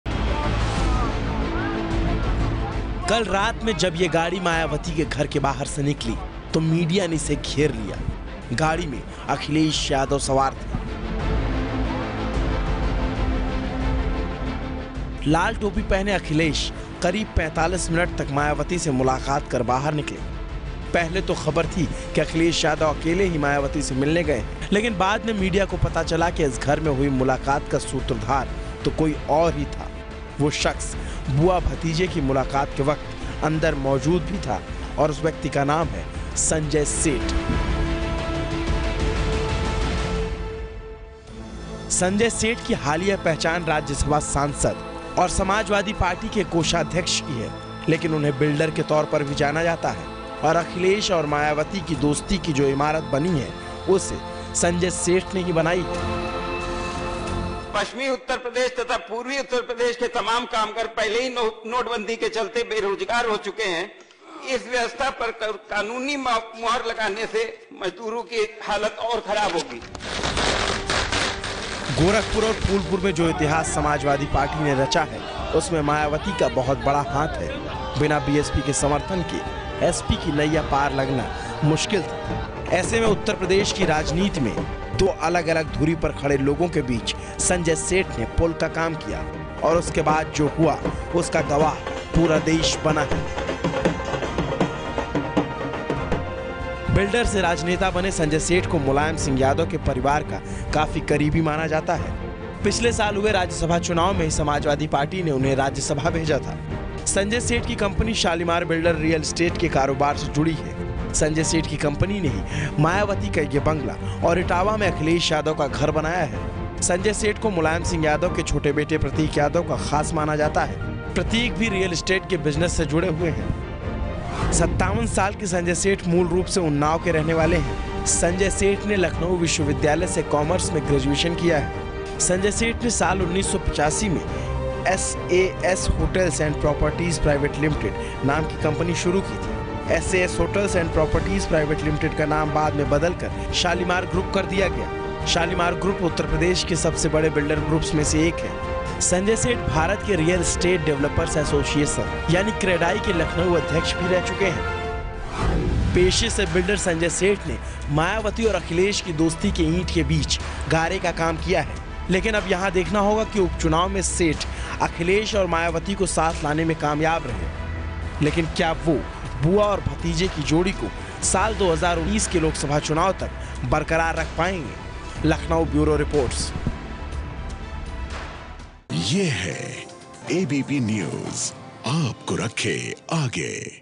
Know about Sanjay Seth, the man behind SP-BSP alliance
Another face that was seen during the 45-minute meeting of Akhilesh yadav and Mayawati was of Sanjay Seth. In a turn of political events in Uttar Pradesh after Samajwadi Party took both the Lok Sabha bypolls, carrying a bouquet in hand, SP party President Akhilesh Yadav on Wednesday night went to the residence of BSP chief Mayawati, to thank her for the support.Sanjay Seth was elected to the Rajya Sabha and represents the Samajwadi Party. He is also SP's treasurer and is said to be very close to Mulayam Singh Yadav’s family.